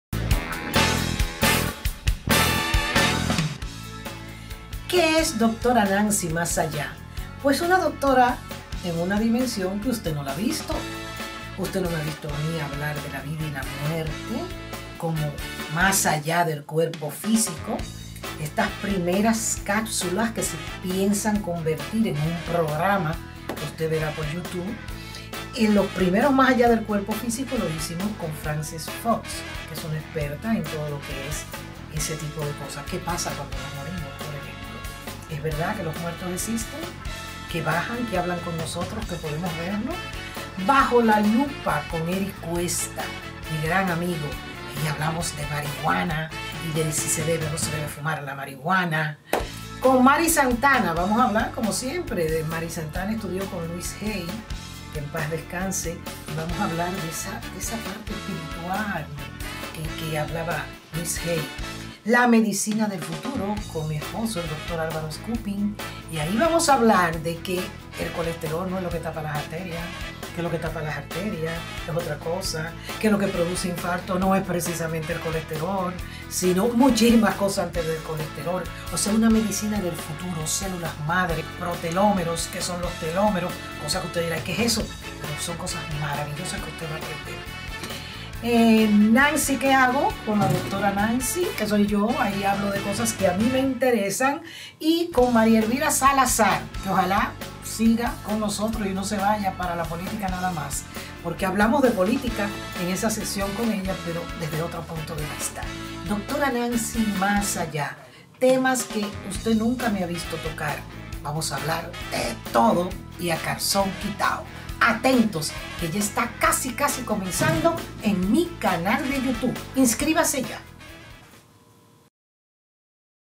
PRÓXIMAMENTE DRA. NANCY MÁS ALLÁ
En su próximo programa Dra. Nancy Más Allá, Ud. podrá verla presentando temas diferentes como nunca la ha visto. Personalidades como Francex Fox, Erick Cuesta, Mari Santana, el Dr. Álvaro Skupin y la Medicina del Futuro, María Elvira Salazar, y la Propia Dra. Nancy, les presentarán temas interesantes y diferentes. Espérela en éste, su canal de Youtube Nancy Alvarez 51. MUY PRONTO!